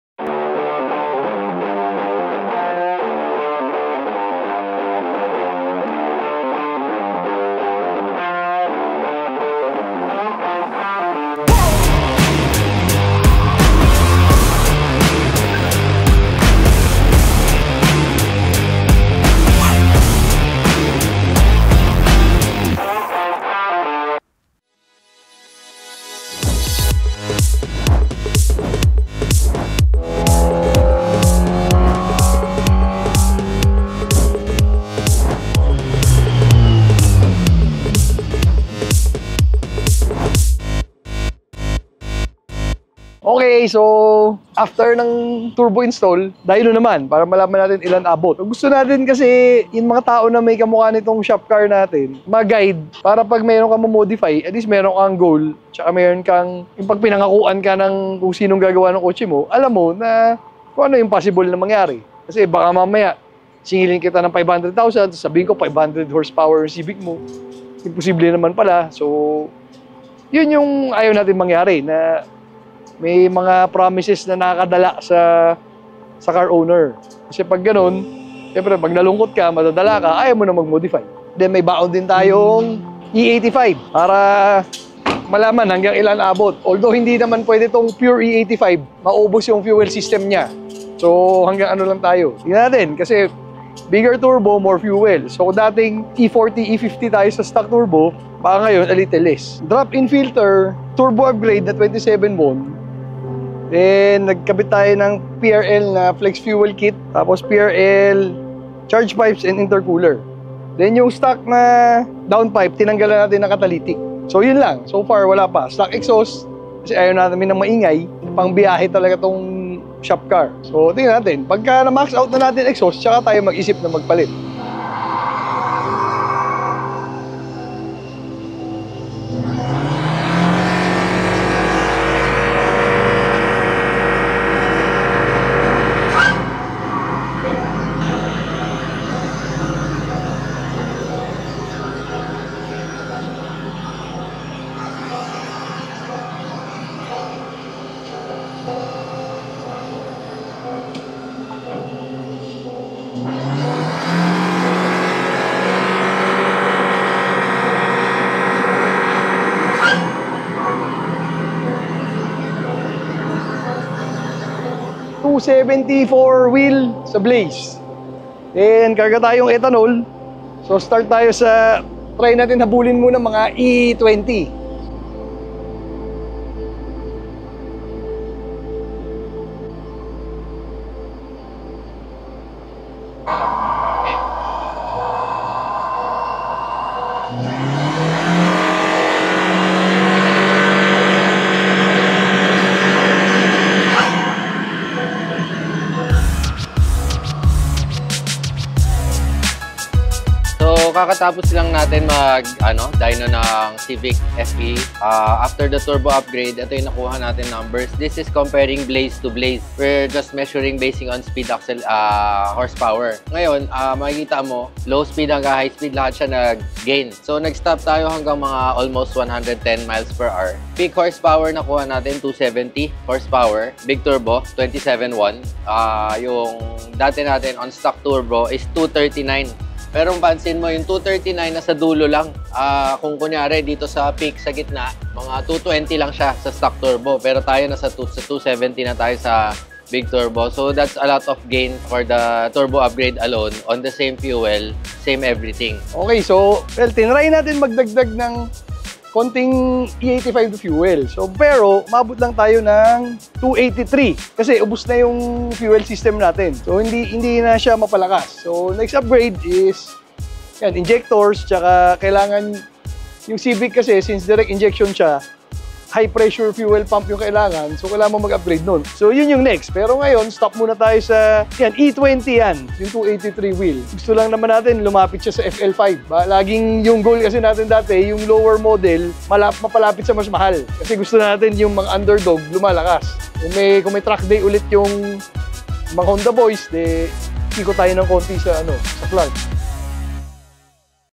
283 WHP CIVIC for less than 300k
Less than 300k pesos for almost 300 Wheel horsepower from your Civic.
Mods list
27Won Turbo and rammer
Prl Intercooler
Decatted stock downpipe
Hondata SWE custom tune
PRL flex fuel kit